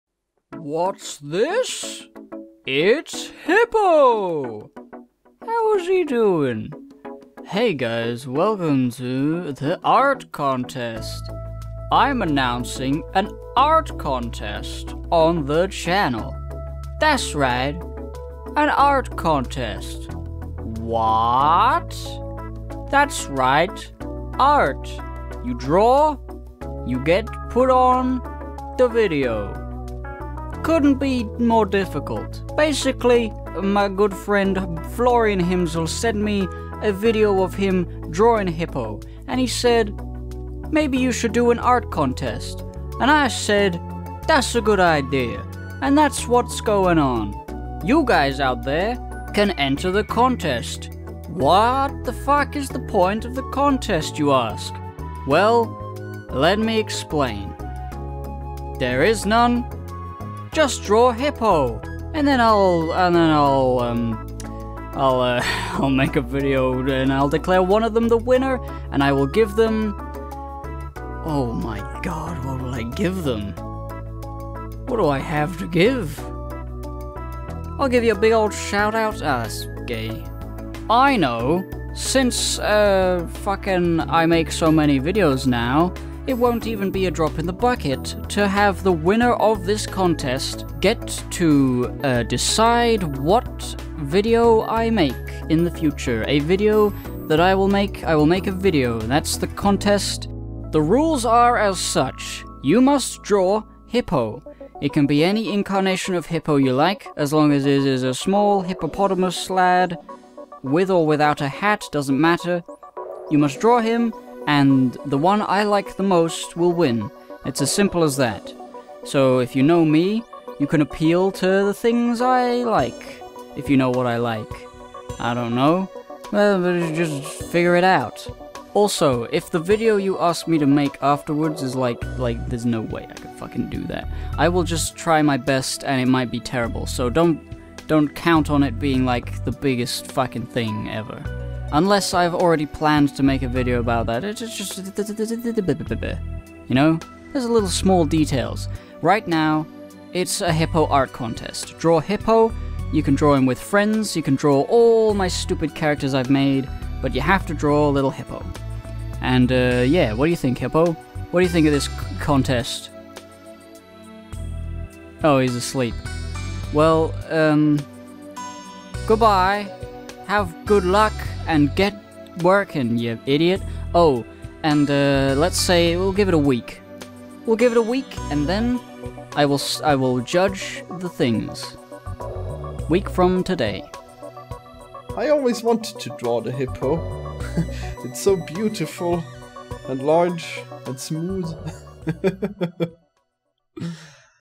HIPPO ART CONTEST
Draw hippo, my favourite one will win, and the winner can choose a topic for a video i will make in the future

Follow the blog to be updated on new videos in case youtube screws up